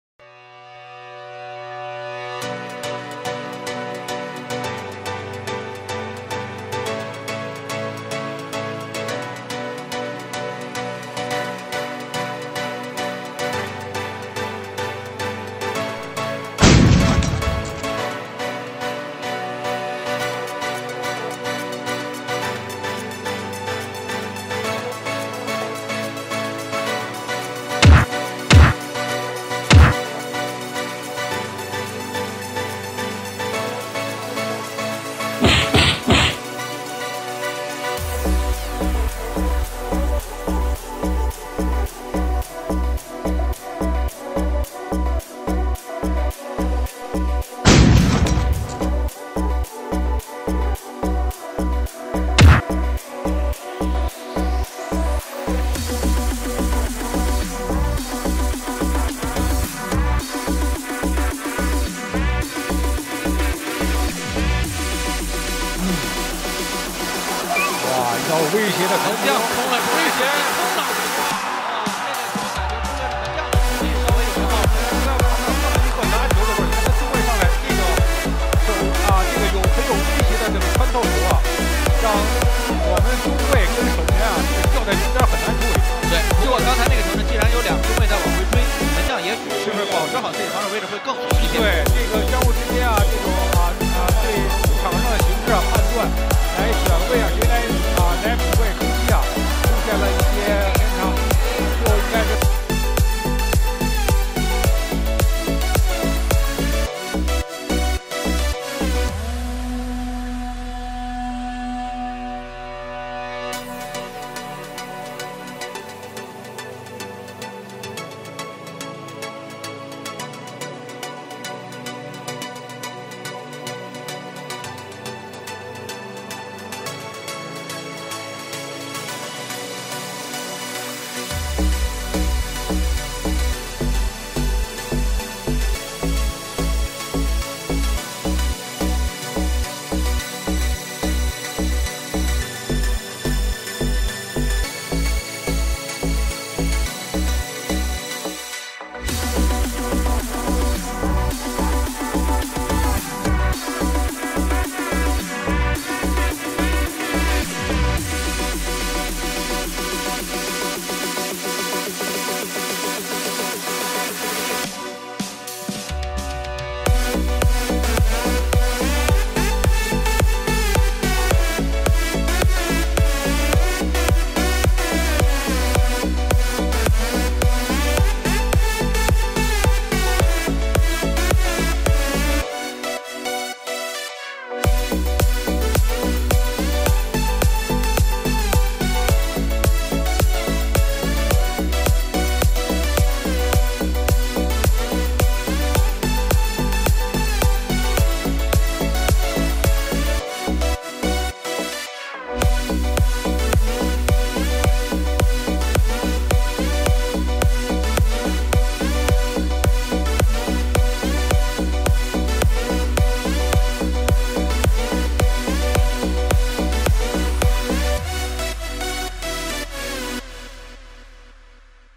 顾超，被中国足协终身禁赛，难道国家队比赛也收钱？| 中国国家队门将失误集锦！
顾超，中国国家队门将失误集锦！2024 年 9 月 10 日，由于顾超参与赌球风波，操纵比赛，被中国足协终身禁赛！